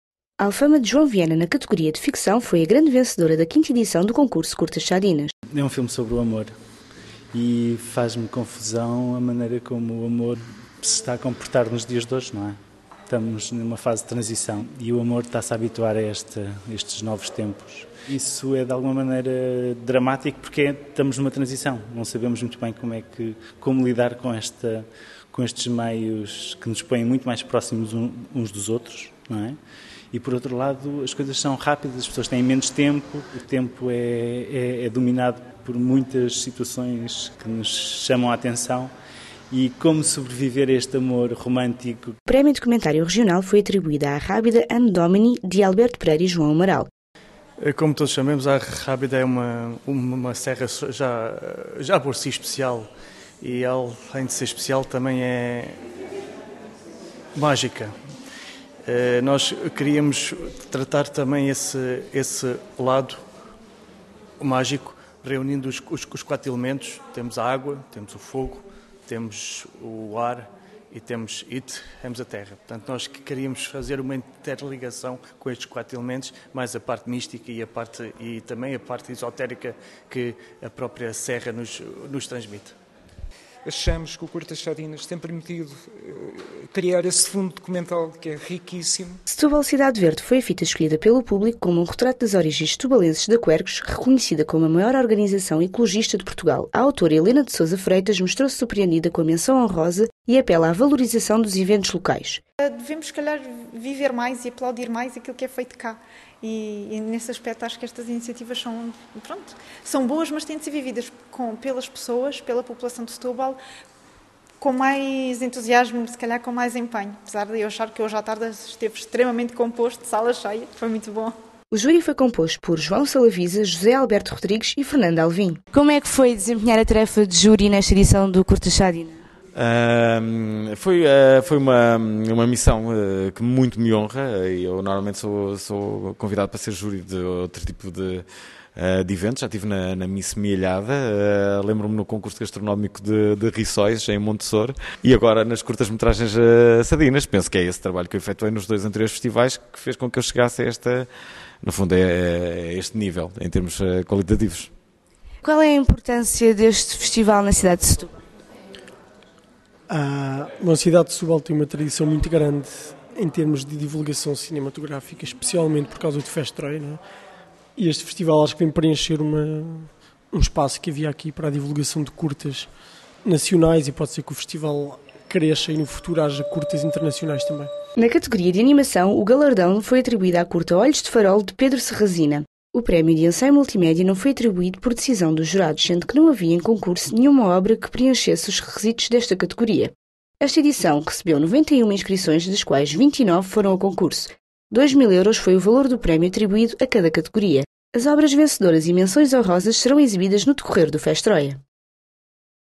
SETUBAL TV - João Viana vence Curtas Sadinas com "Alfama"
A 4ª edição do Festival Curtas Sadinas premiou "Alfama", de João Viana, na Categoria de Ficção, "Arrábida - Anno Domini", de Alberto Pereira e João Cabral, em documentário. "Ode Difícil" conquistou o prémio escolar, enquanto "Os olhos do Farol" foi o vencedor na animação! Helena de Sousa Freitas, com o documentário "Setúbal, cidade verde" foi a vencedora do prémio do público. (16-05-11)